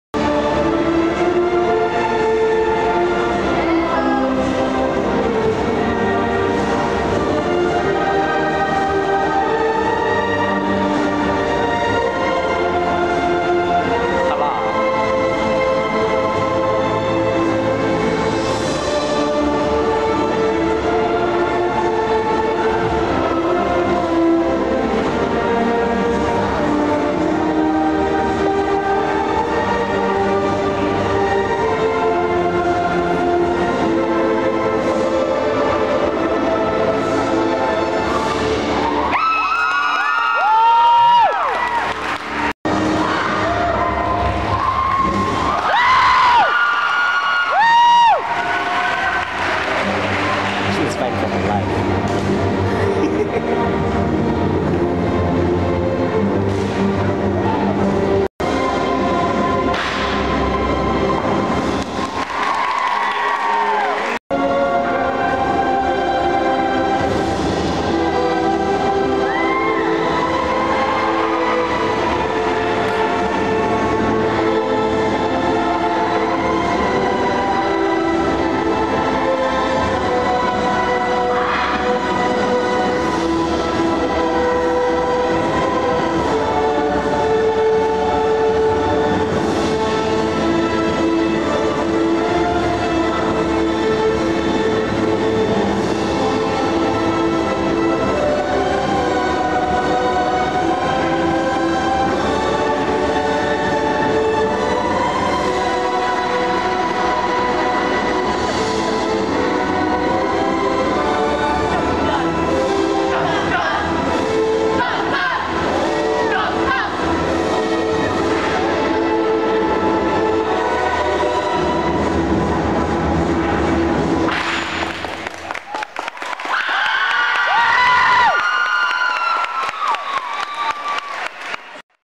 Hogan Performance Ensemble 2015 - Championships @ Independence (CLIPS)
as it says in the title, there are only chunks of the show here. sorry! i was using my camera for other shows and the whole day in general :(